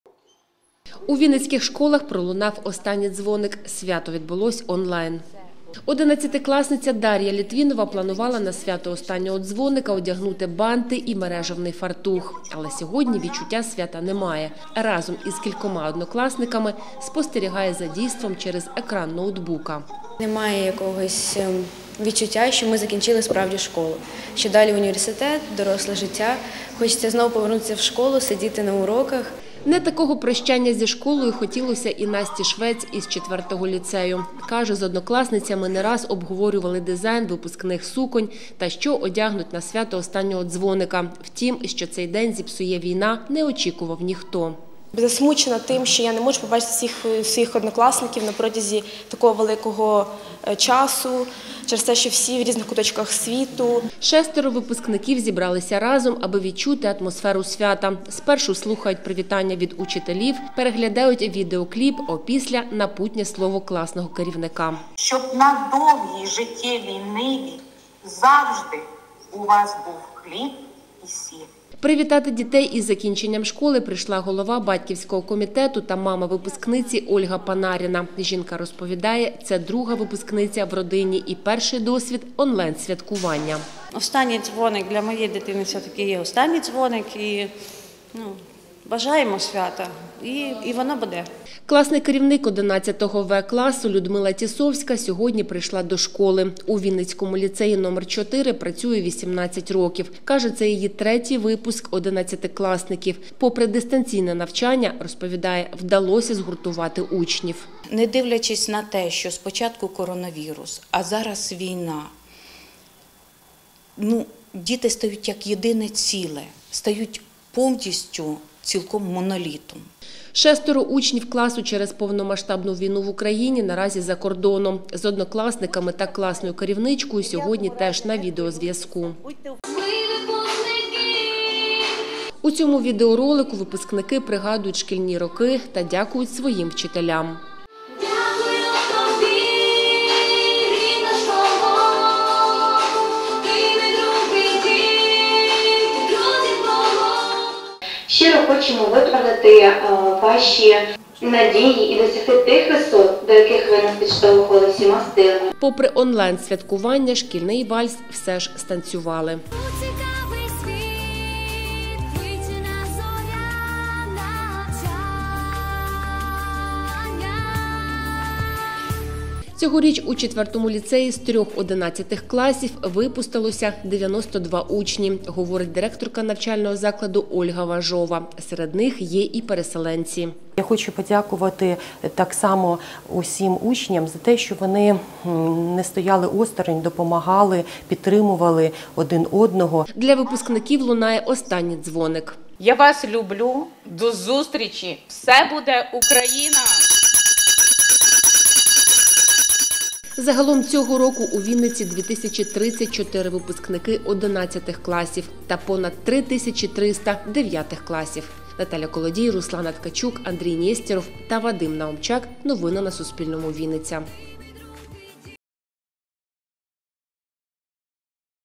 Свято онлайн: у Вінниці пролунав останній дзвінок
У вінницьких школярів пролунав останній дзвоник. Свято відбулось онлайн. Цього вимагають воєнні реалії, – каже директорка міського департаменту освіти Оксана Яценко. Цьогоріч у Вінниці 2034 випускники одинадцятих та понад три тисячі триста дев'ятих класів.